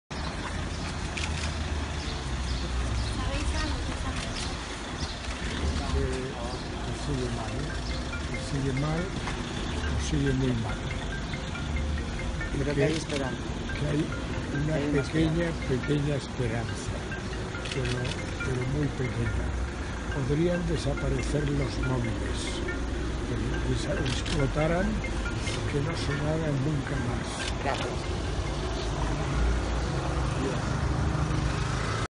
Declaraciones de Ricardo Urgell sobre Ángel Nieto a la salida de la clínica
Ricardo Urgell ha explicado a su salida de la Clínica Vilás que Ángel Nieto sigue muy mal, pero hay esperanza. "Hay una pequeña, pequeña esperanza, pero muy pequeña", ha comentado Urgell con tristeza.